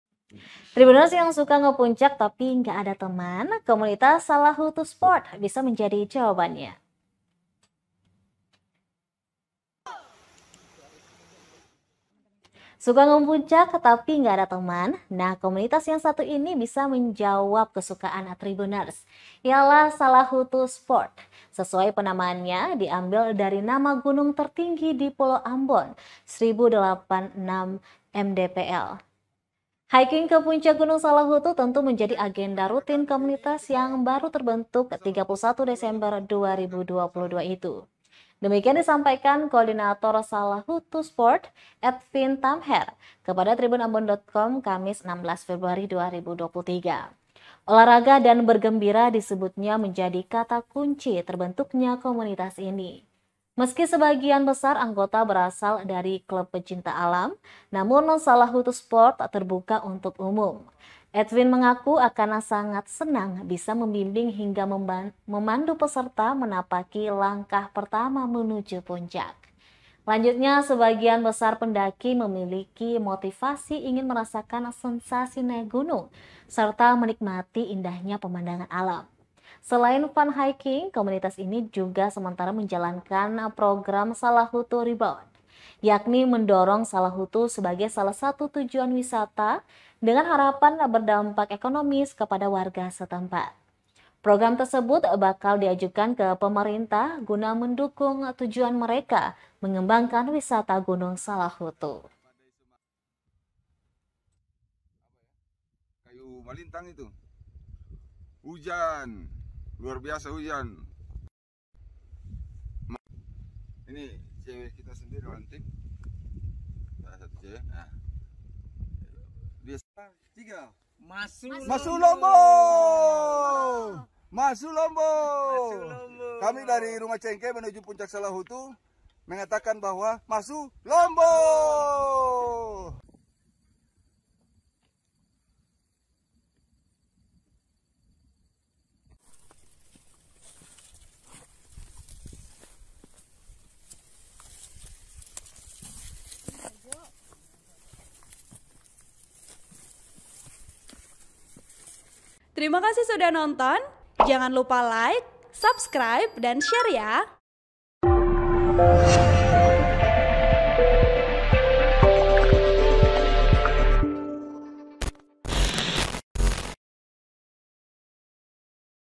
Cari Teman Hiking? Gabung Aja ke Komunitas Salahutu Sport
ED, Laporan Wartawan TribunAmbon.com, Jenderal Louis

AMBON, TRIBUNAMBON.COM - Suka ngepuncak, tapi gak ada teman?

Nah, komunitas yang satu ini bisa jadi menjawab kesukaan Tribunners.

Ialah Salahutu Sport.

Sesuai penamaannya, diambil dari nama gunung tertinggi di Pulau Ambon; 1.086 mdpl.

Hiking ke puncak Gunung Salahutu tentu menjadi agenda rutin komunitas yang baru terbentuk 31 Desember 2022 itu.

Demikian disampaikan Koordinator Salahutu Sport, Etvin Tamher kepada TribunAmbon.com, Kamis (16/2/2023).

Olahraga dan bergembira disebutnya jadi kata kunci terbentuknya komunitas ini.

Meski sebagian besar anggota berasal dari club pecinta alam, namun Salahutu Sport terbuka untuk umum.

Etvin mengaku akan sangat senang bisa membimbing hingga memandu peserta menapaki langkah pertama menuju puncak.

Lanjutnya, sebagian besar pendaki memiliki motifasi ingin merasakan sensasi naik gunung serta menikmati indahnya pemandangan alam.

Selain Fun Hiking, komunitas ini juga sementara menjalankan program Salahutu Rebound.

Yakni; mendorong Salahutu sebagai salah satu tujuan wisata dengan harapan berdampak ekonomis kepada warga setempat.

Program tersebut bakal diajukan ke pemerintah guna mendukung tujuan mereka mengembangkan wisata Gunung Salahutu.